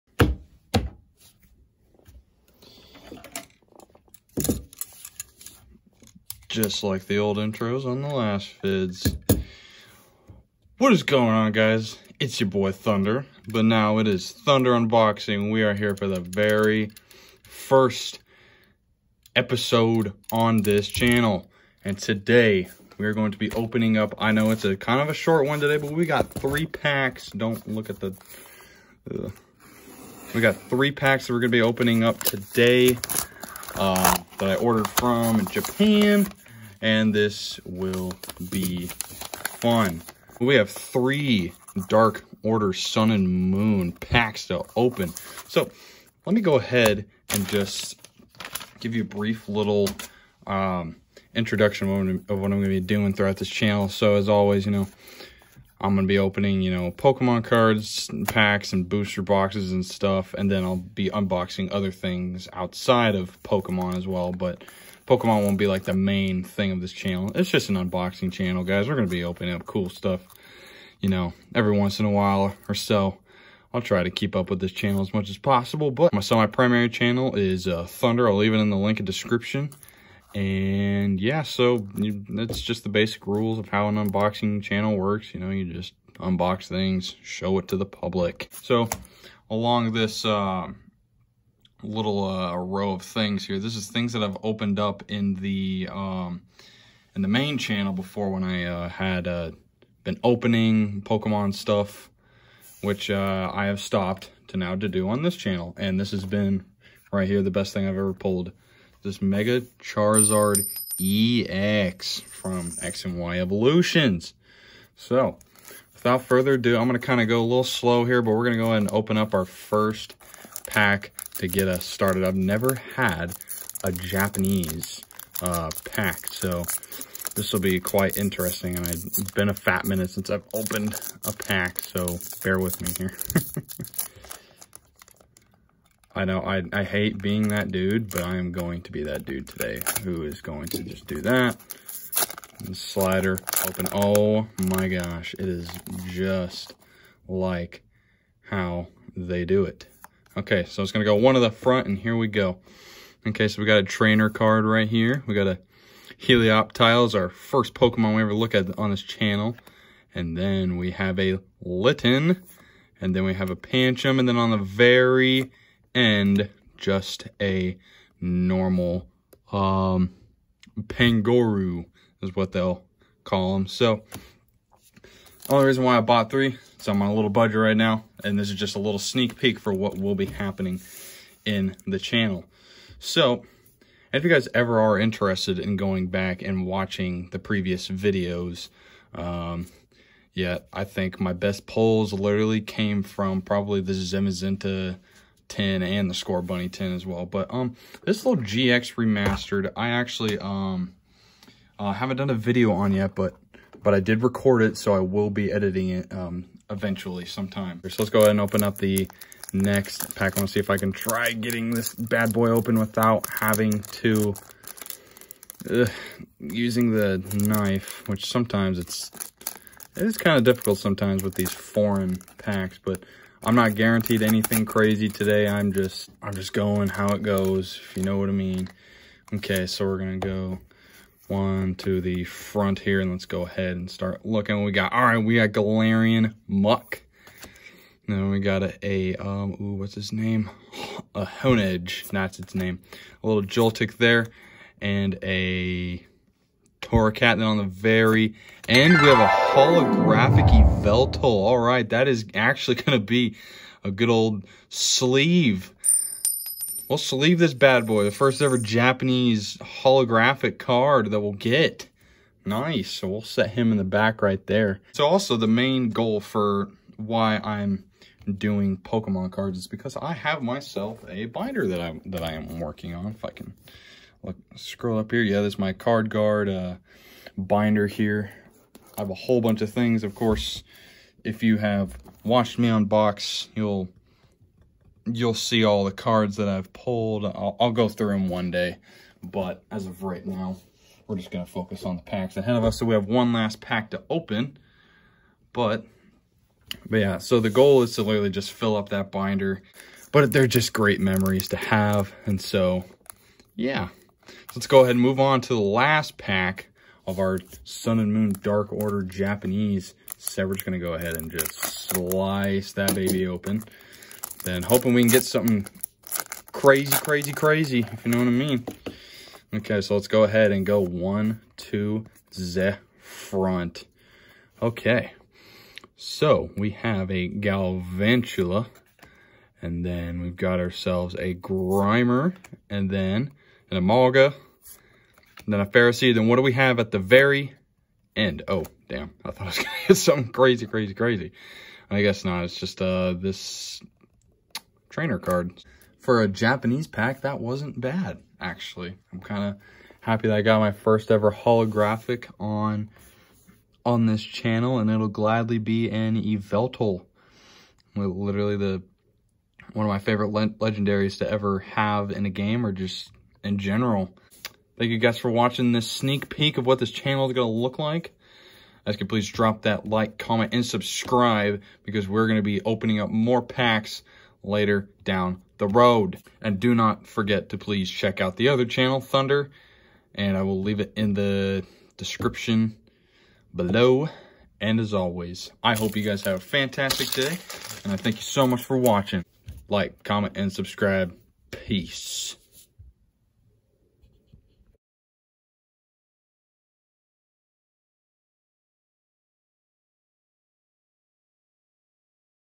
FIRST OPENING!! 3 Sun and Moon Dark Order Packs!
hey guys! Welcome to the very first episode of this channel! I am excited to start an adventure with you guys and I am ready to unbox more things!